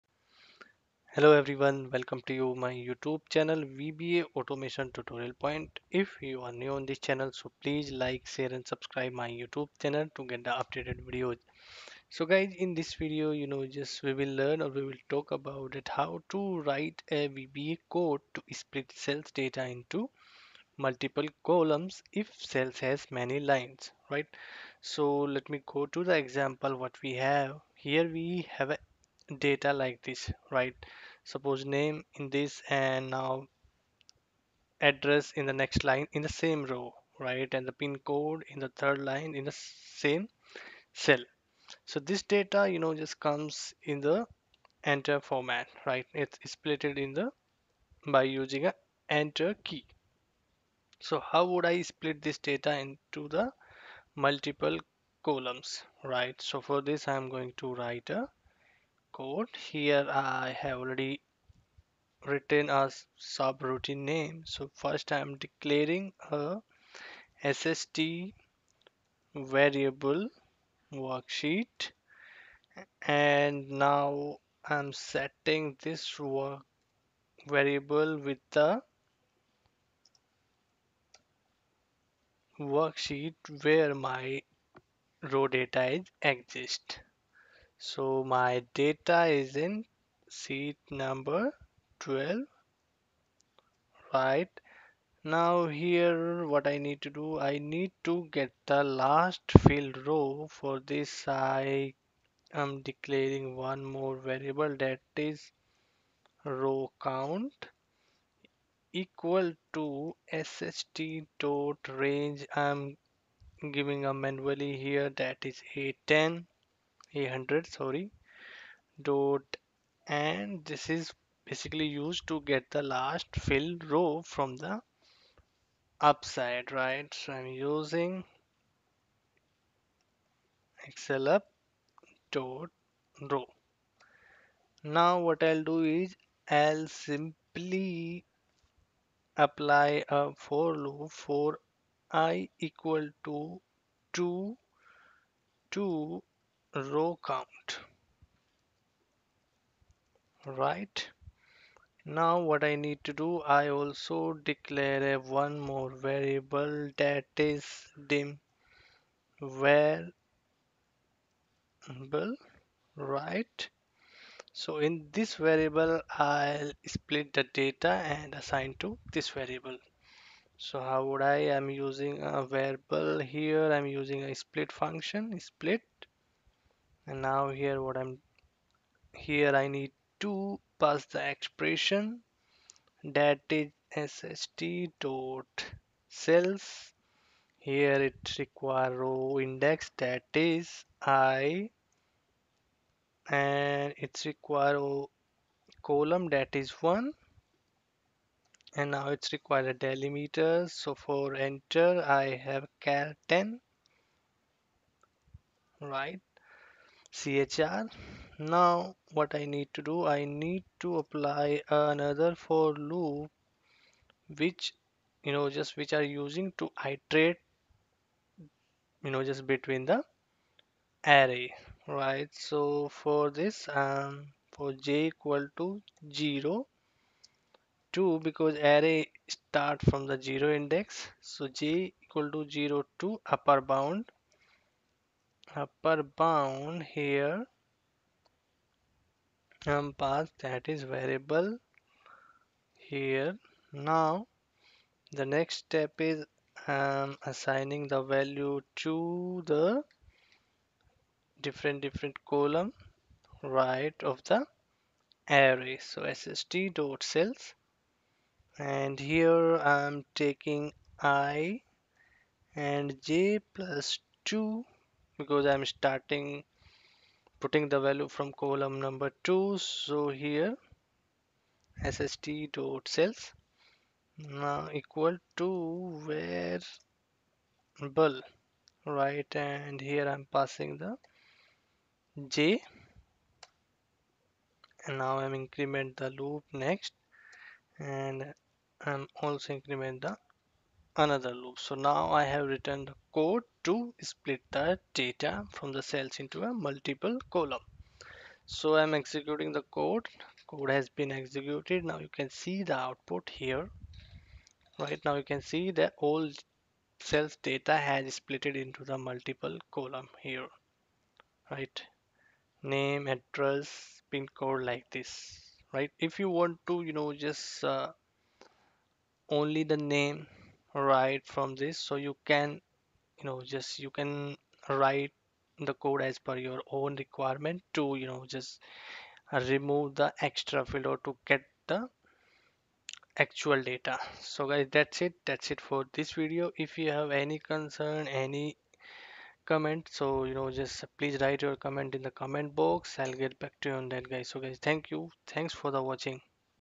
VBA code to split cells data into columns if cell has many lines.
vba code to split cell data into columns if cell has many lines or data is stored in cells by the new line or enter key chr 10#